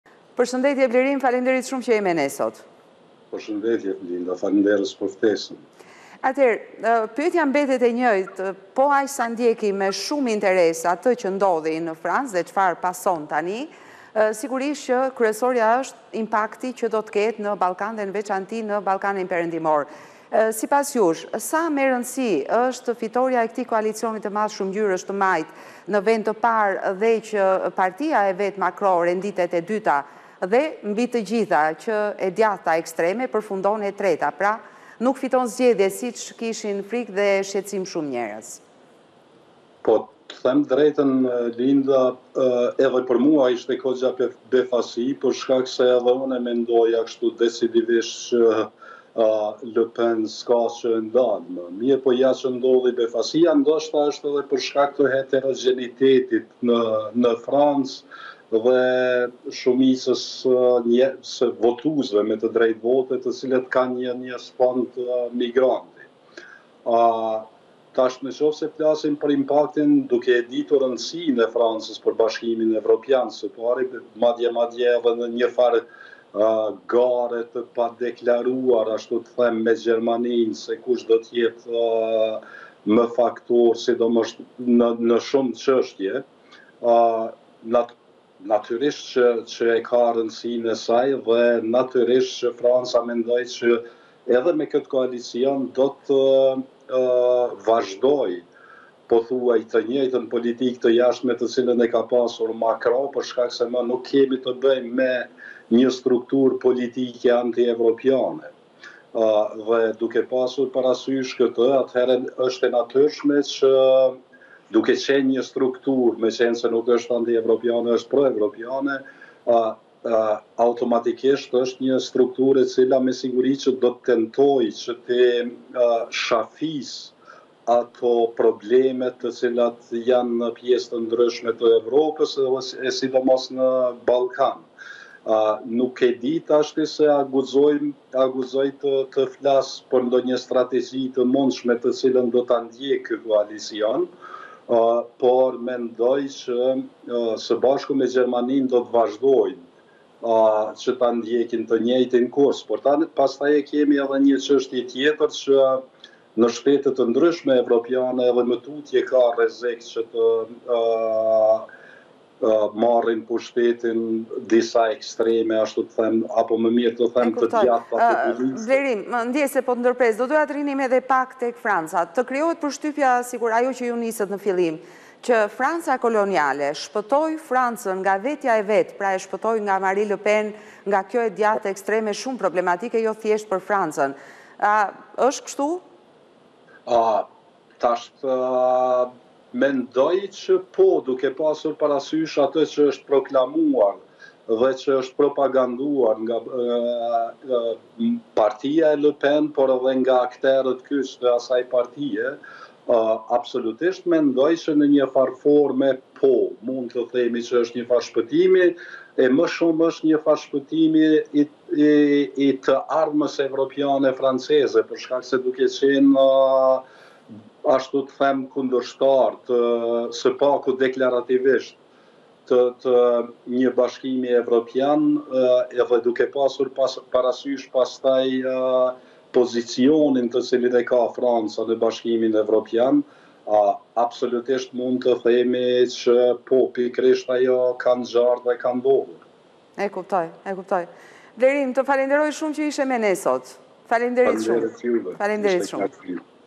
Zgjedhjet parlamentare në Francë/ Canaj: Ishin befasi, shkak mund të jetë heterogjeniteti në vend
Ballkan, gjithcka për Ballkanin, në Ballkan. Politike, ekonomi, kulture, lufte dhe paqe! Një emision për Ballkanin dhe ballkanasit!Emision nga Linda Karadaku